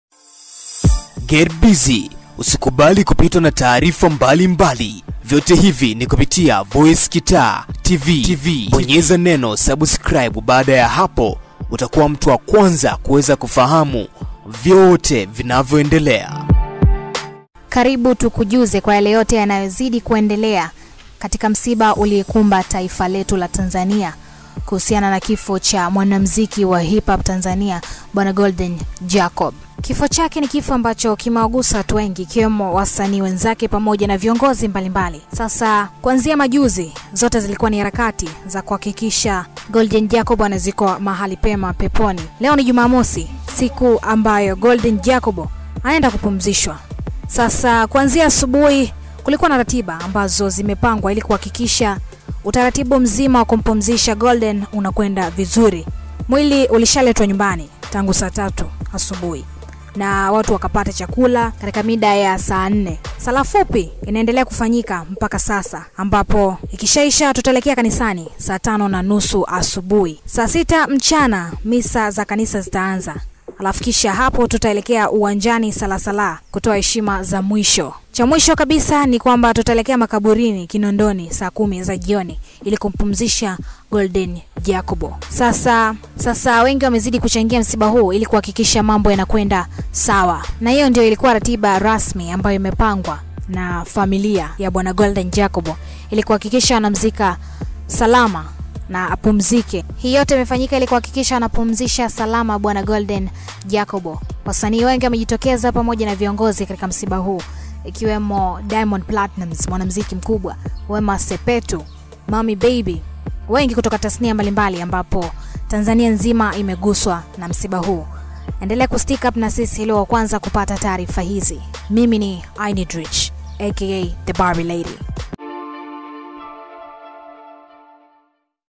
GODZILLA KUZIKWA LEO MAKABURI YA KINONDONI TAZAMA LIVE KUTOKA MSIBANI
Msanii wa hip hop Godzilla kuzikwa leo alale pema peponi